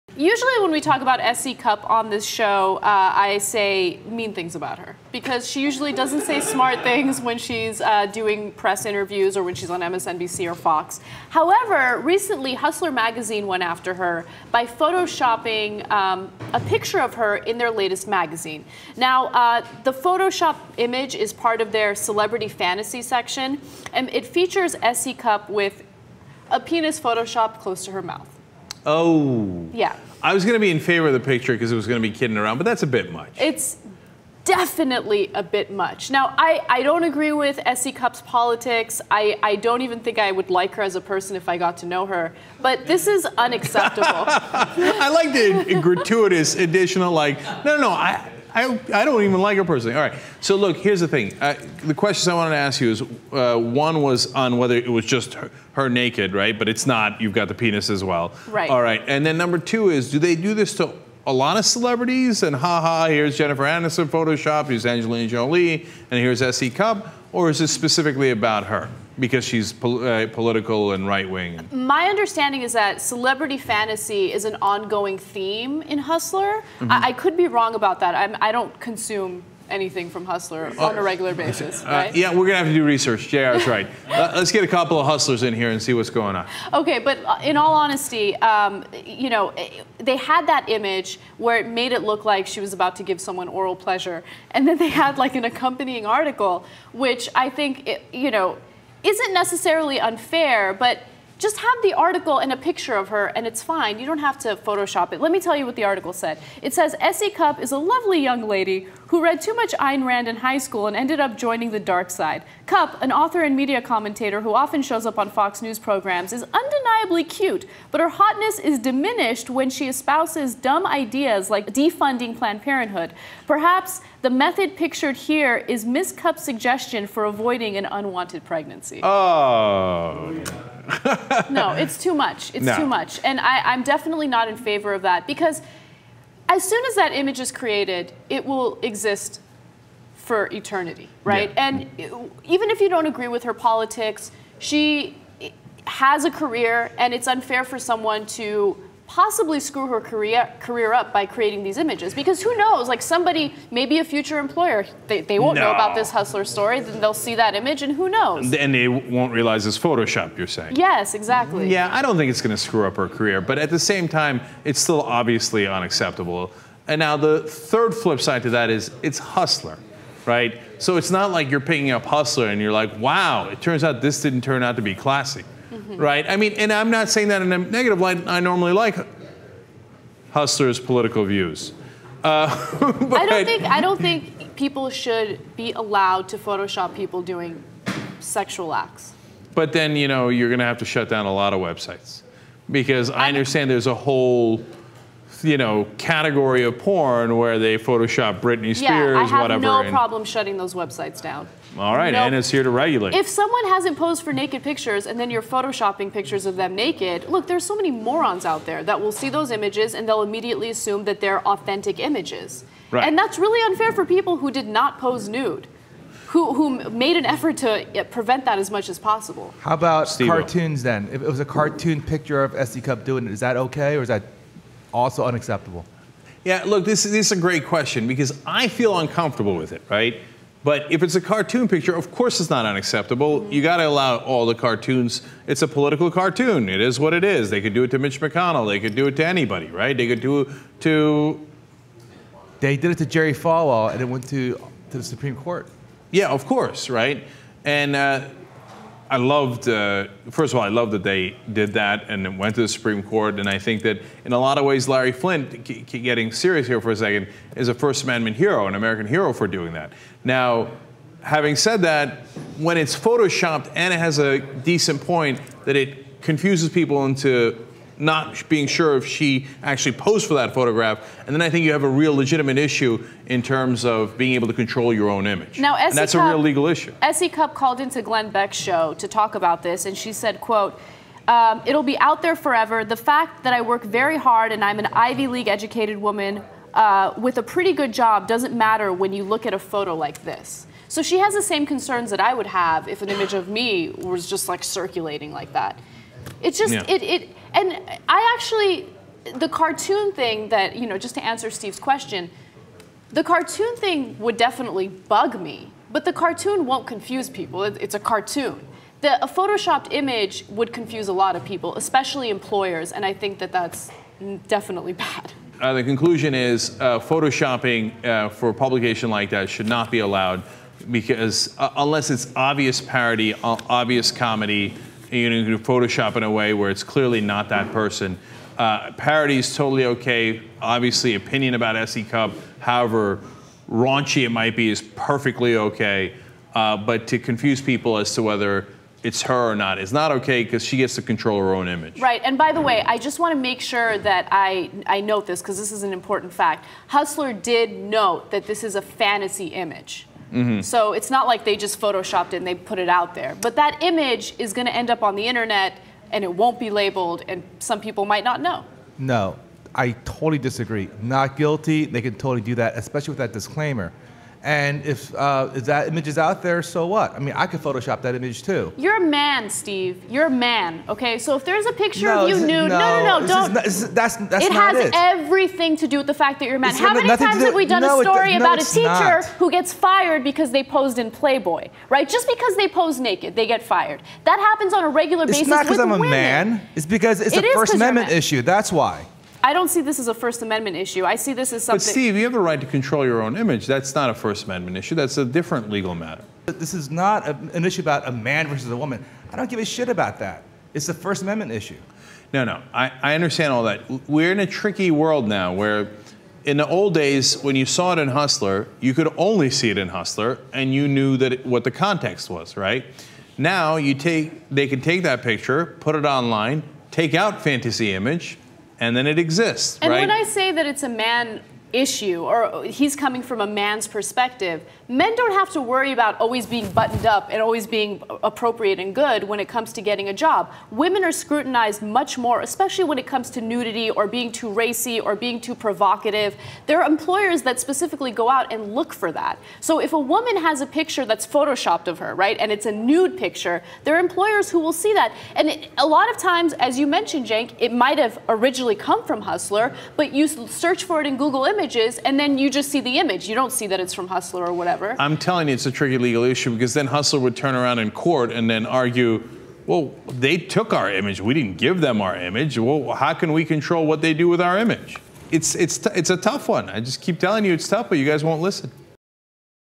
Hustler Photoshopped X-Rated S.E. Cupp's Image
In a recent issue of Hustler Magazine a photoshopped x-rated image of Fox News contributor S.E. Cupp was published along with a story about her opinions.